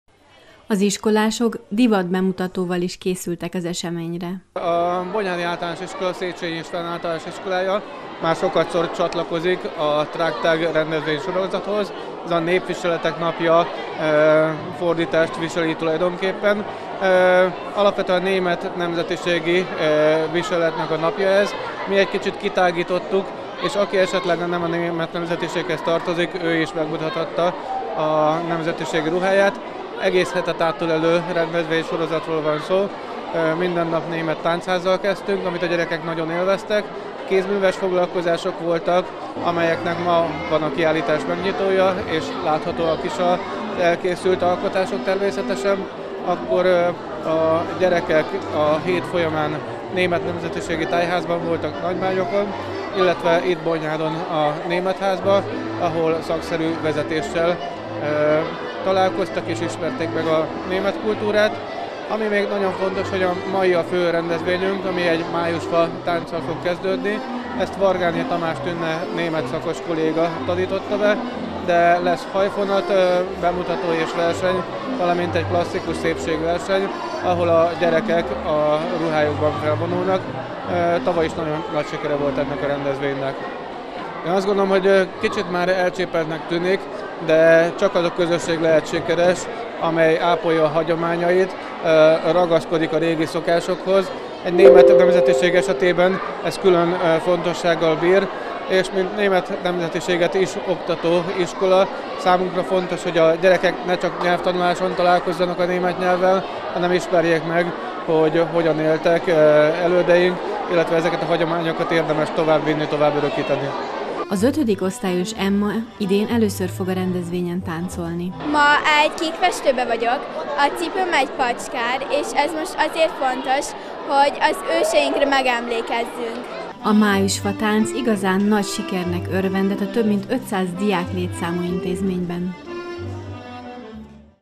Bonyhádi Általános Iskola - a népviselet napját ünnepelték
A Bonyhádi Általános Iskola a Népviselet Napja alkalmából rendezett programokat. Az intézmény vezetője és tanárai fontosnak tartják, hogy a diákok a hagyományokkal is foglalkozzanak.